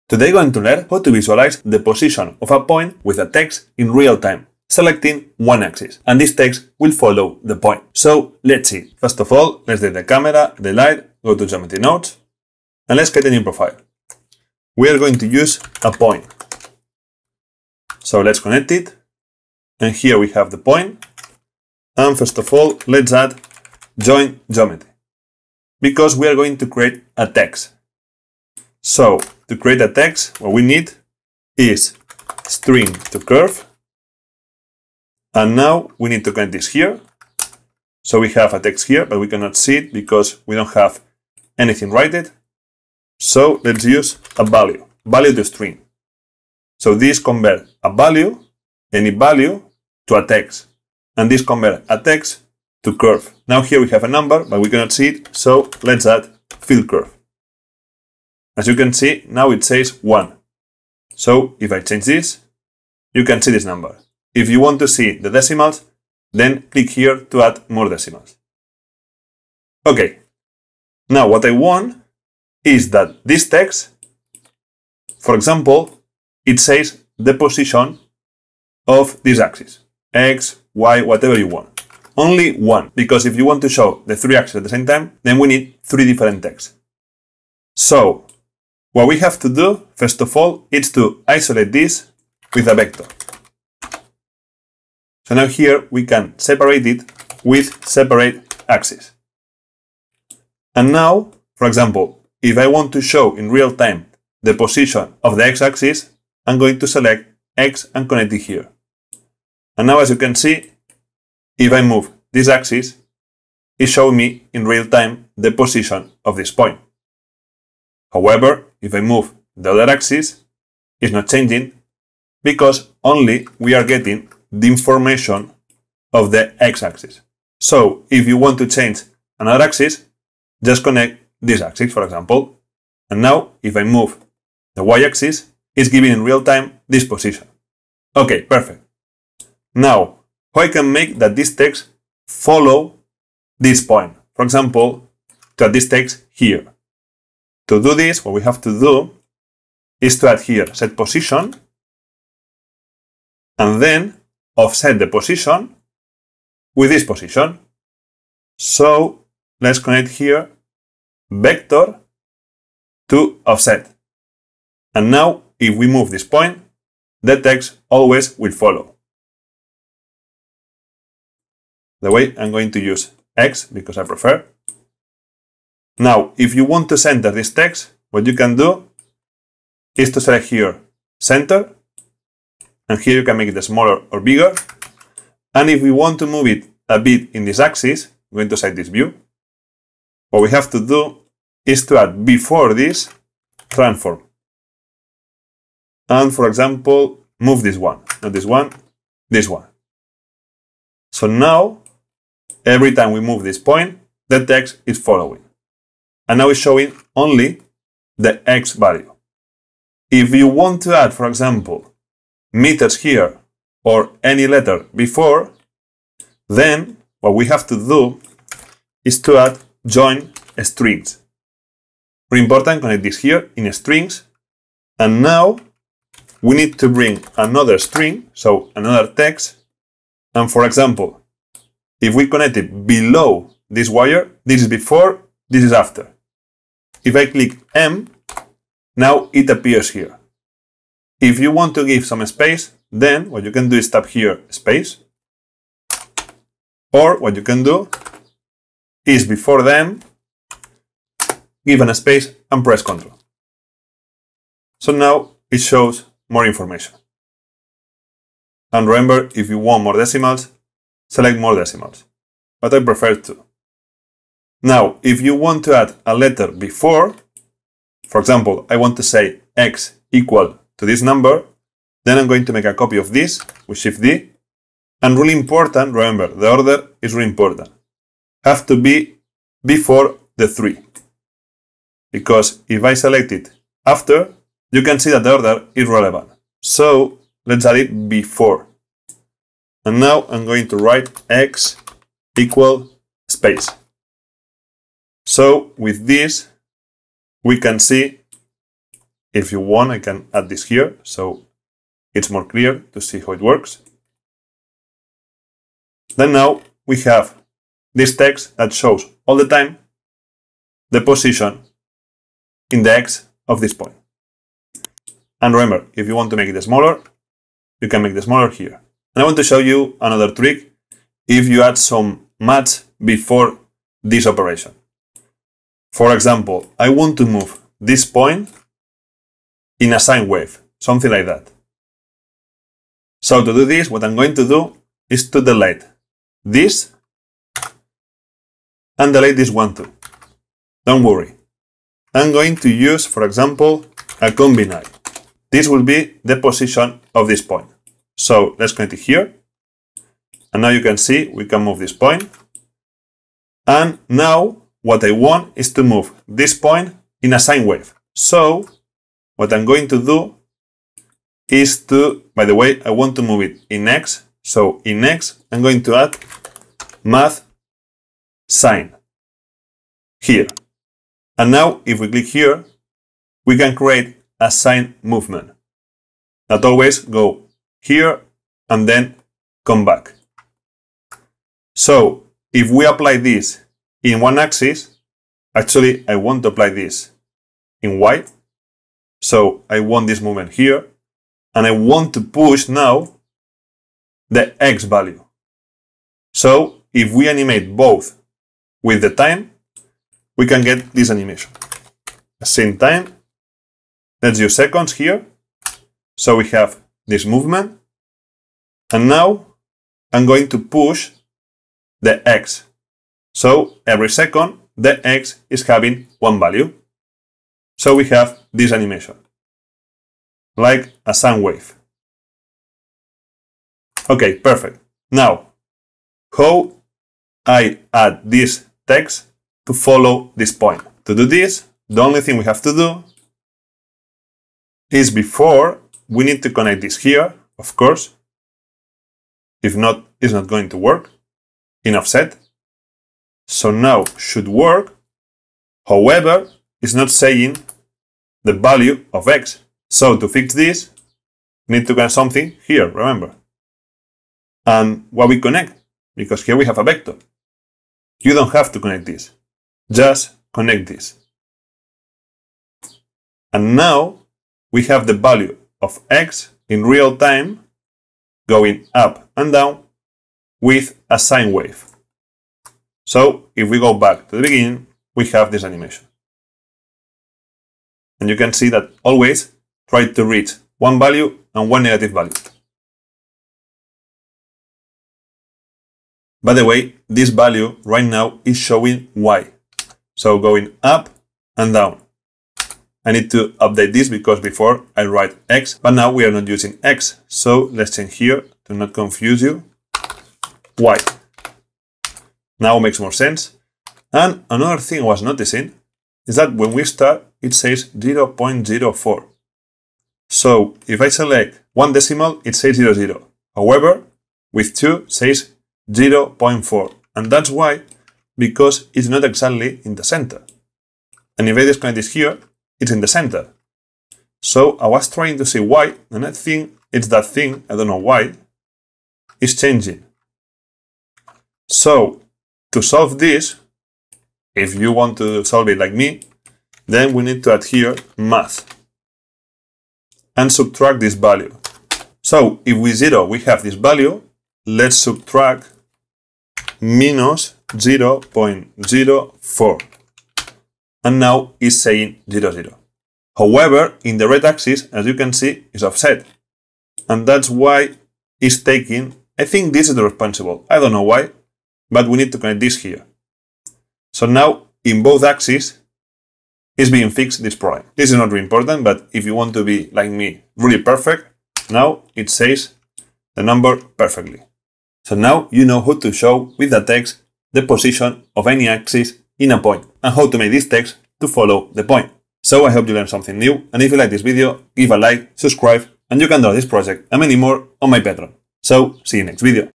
Visualize TEXT POSITION in Real time in Blender - Geometry Nodes
Learn how to visualize an axis position of a point in real-time with a text. All done with Geometry Nodes in Blender.

00:00 Intro
00:18 Create a text
01:09 Text shows an axis value
02:18 Text follow a position
03:30 Add letters before and after a value
05:19 Adding maths operations
08:24 Fixing Sine Wave initial position